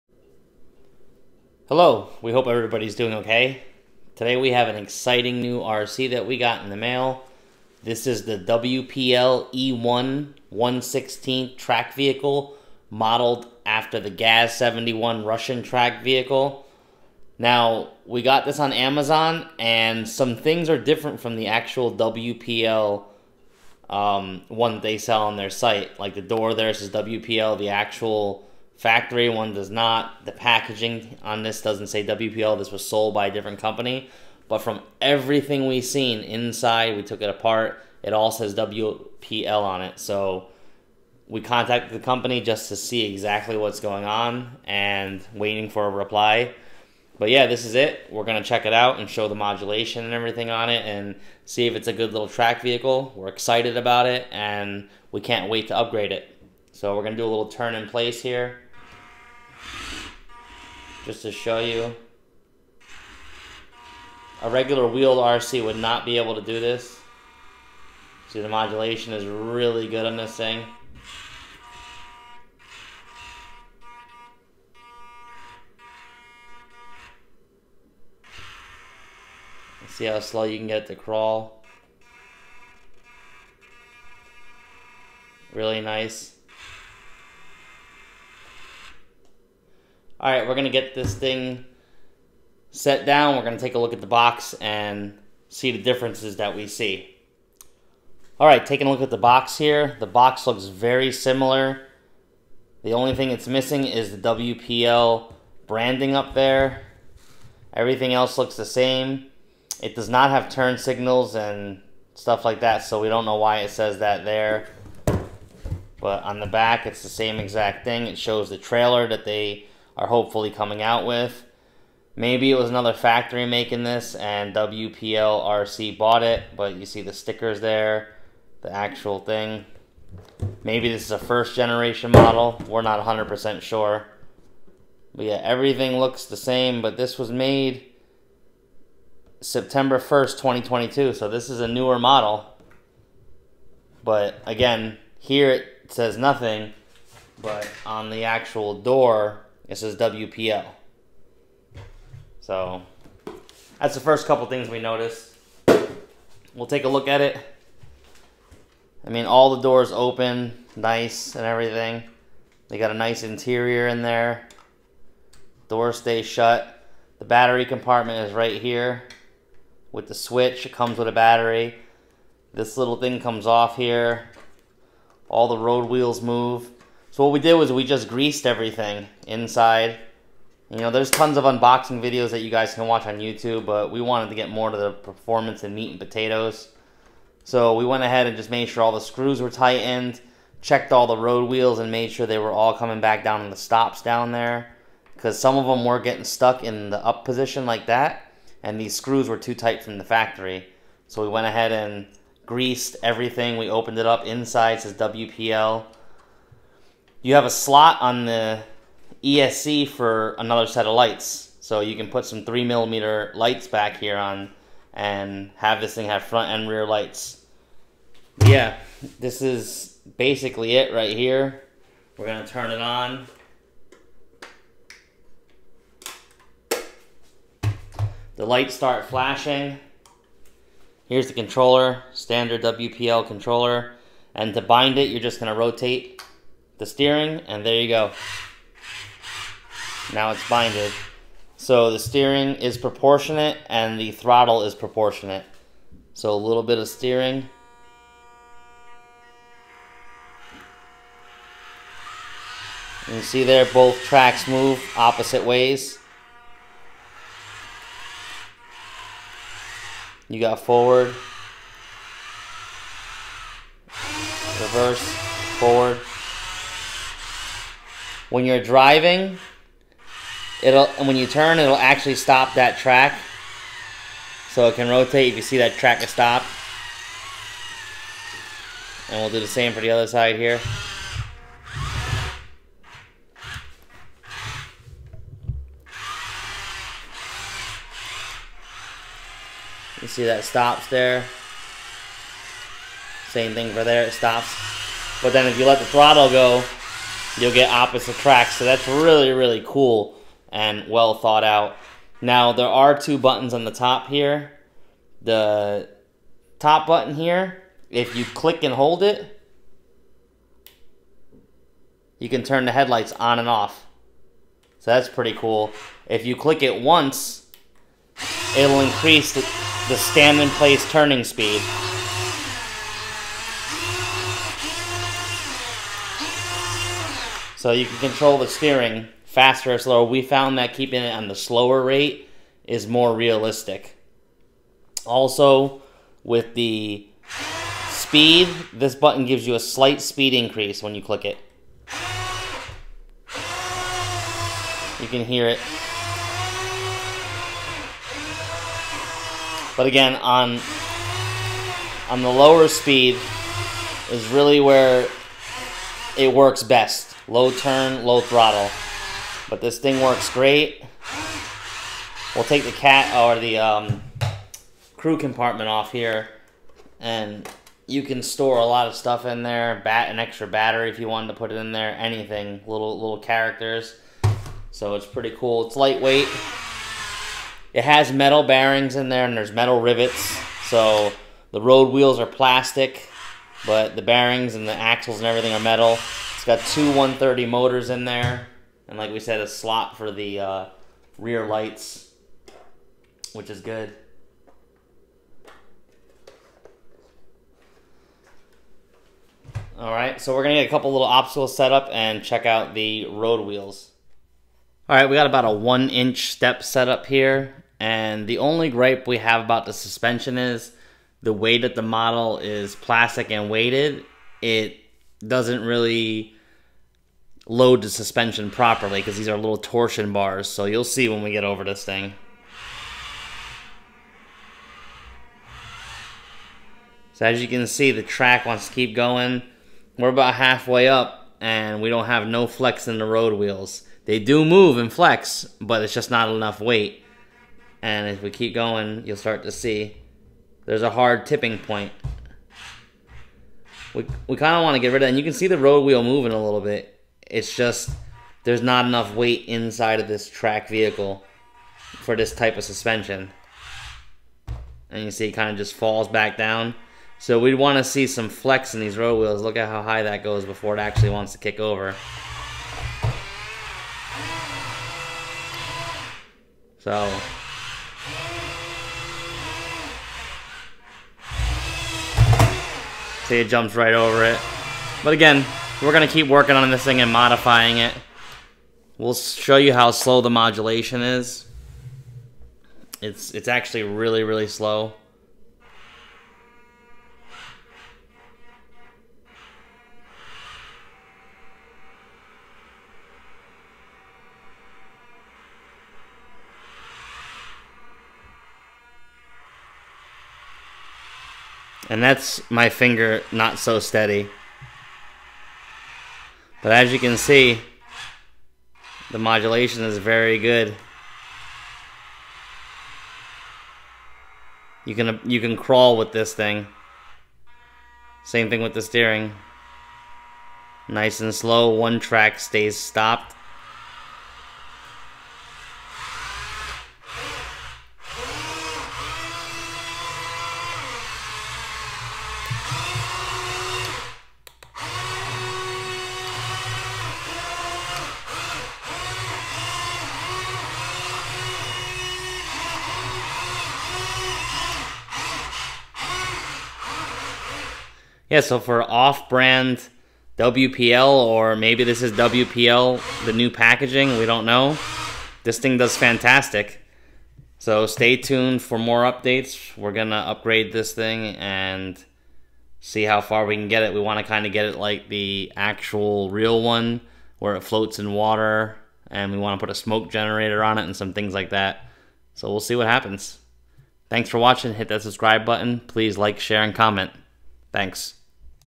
WPL RC E1 Tracked Vehicle GAZ-71 (Fisca Remote Control Crawler Military Track Transport Truck, 1/16)
We found this on Amazon for an AMAZING price this is the say Exact WPL E1. Functions flawlessly!

Info:

RC Crawler Transport Car: This military truck equipped with tracks can go forward / backward, turn right / left, turn round. It’s pretty suitable for running off-road terrain outdoors.
Proportional Throttle & Steering Control: The proportional throttle and steering control function in the sensitive remote control system allows players to have conditions to control the speeds and fine tune direction by controller.
2.4Ghz High Frequency: Automatic pairing, long control distance, good anti-jamming capability. Suitable for more trucks playing at the same time without Interference.
High Quality Materials: Bodywork is made of ABS plastic; Tracks are made of rubber. It is durable enough for indoors and outdoors playing all terrain.
Great Gift for Kids: 7.4V 500mah rechargeable battery (Included) can provide 15 - 20 mins playing time, charging time 2 hours. It comes with some accessories to DIY. It is a perfect birthday, Christmas gifts for kids.

Fisca RC crawler transporter is a great toy for kids aged 6+ years old, Featured Proportional Throttle & Steering Control Function it will provide much fun to children while they playing it .

Highlights:
--- Proportional Throttle & Steering Control: Allow player to control the speeds and fine tune direction.
--- 2.4Ghz High Frequency: Suitable for more trucks playing at the same time without Interference.
--- High Quality Materials: Made of durable plastic and rubbers, It’s durable enough for indoors and outdoors playing all terrain.
--- Large Rechargeable Battery: Provide 20 mins playing time after fully charged, just needs 2 hours to charge.
--- Comes with Accessories to DIY: It comes with many little accessories, you can DIY the car, and its cover of the cabin is detachable.

Specification:
Age for: 6+ Years Old
Weight: 2.03 lb
Materials: Plastic; Rubber
Remote Frequency: 2.4Ghz
Remote Range: 100 feet
Battery Capacity: 7.4V 500mAh
Working Time: 20 mins
Charging Time: 2 Hours
Dimensions: 11.4 * 5.5 * 3.9 inches

Package List
1 x Transporter
1 x Controller
1 x Battery
1 x USB Cord
1 x Pack of Accessories